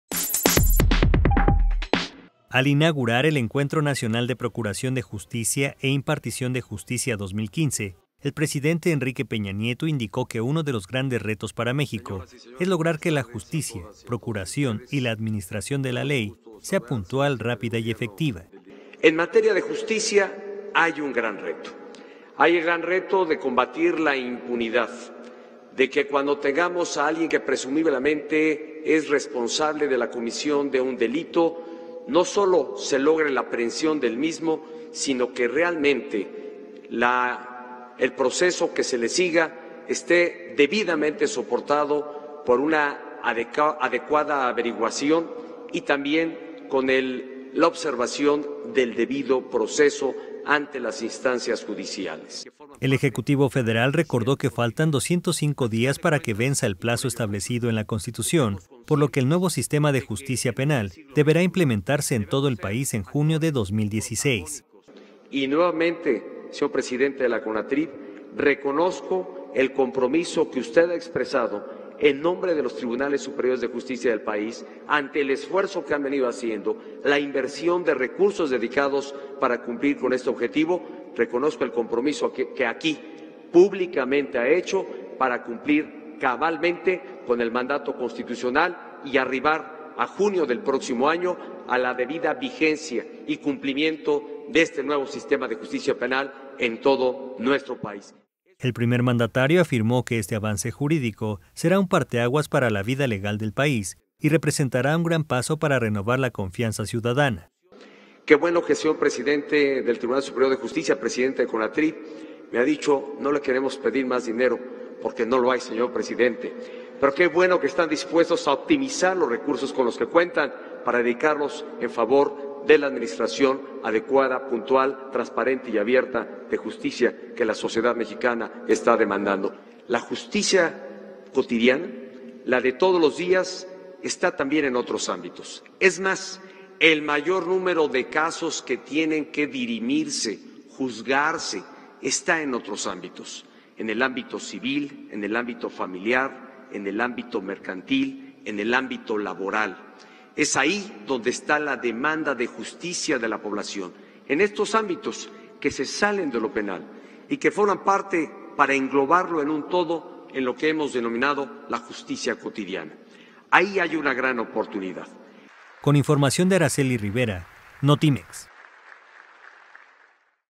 El Nuevo Sistema de Justicia Penal será un parteaguas para México
El Presidente Enrique Peña Nieto recordó que en junio de 2016 debe estar implementado este sistema en todo el país